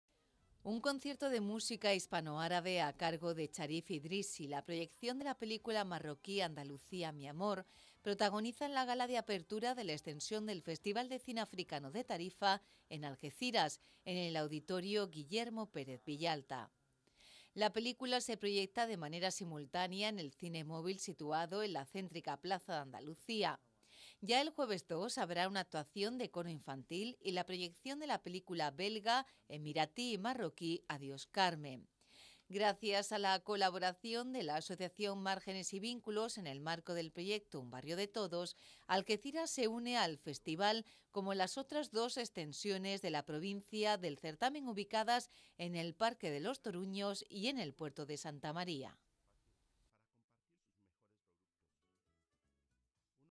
El Festival de Cine Africano de Tarifa llega a Algeciras
La nueva extensión del Festival de Cine Africano de Tarifa en Algeciras tiene esta tarde su gala de apertura. Tras el pasacalles de ayer, que desde el Parque María Cristina hasta la Plaza de Abastos recorrió las calles del centro, el FCAT celebra hoy a las ocho de la tarde en el edifico Guillermo Pérez Villalta la segunda de las actividades en la ciudad, que contará con presencia del certamen hasta el jueves.